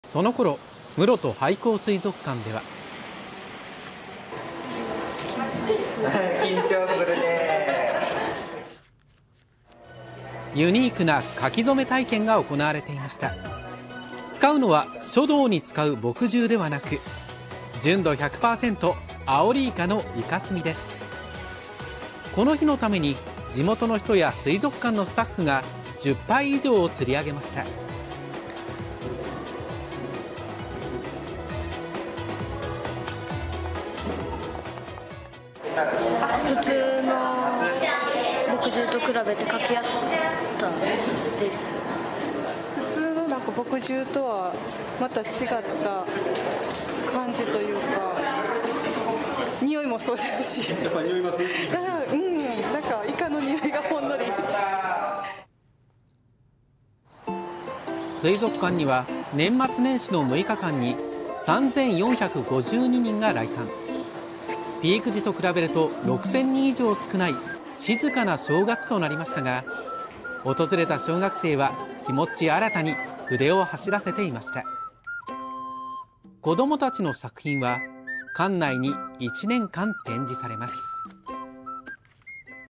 【新春スケッチ】墨汁じゃない純度100%！廃校水族館で“イカした”書き初め体験【高知】 (23/01/04 19:50)
むろと廃校水族館では―

徳島から・小4「緊張する…」
スタッフ「緊張するねー」

ユニークな「書き初め」体験が行われていました。
使うのは書道に使う墨汁ではなく…純度100%！アオリイカのイカスミです。
この日のために地元の人や水族館のスタッフが10杯以上を釣り上げました。

徳島から・小4：
「普通の墨と比べて書きやすかったです」

徳島・小学生の母親：
「墨汁とはまた違った感じというかにおいもそうですし」

カメラ「やっぱり違う？」
徳島・小学生の母親「イカのにおいがほんのり…」

水族館には年末年始の6日間に3452人が来館。
ピーク時と比べると6000人以上少ない静かな正月となりましたが、訪れた小学生は気持ち新たに筆を走らせていました。

子供たちの作品は館内に1年間展示されます。